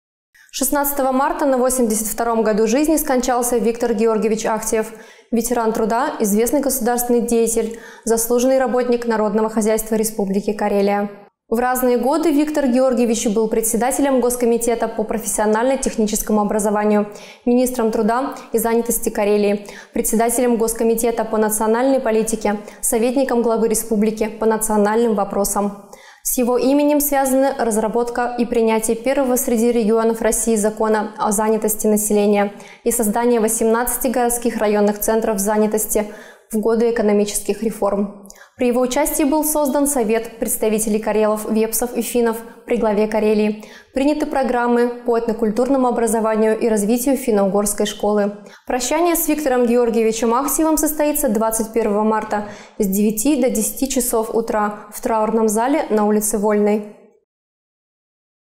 16 МАРТА НА 82-ОМ ГОДУ ЖИЗНИ СКОНЧАЛСЯ ВИКТОР ГЕОРГИЕВИЧ АХТИЕВ
Виктор Георгиевич - ветеран труда, известный государственный деятель, заслуженный работник народного хозяйства Республики Карелия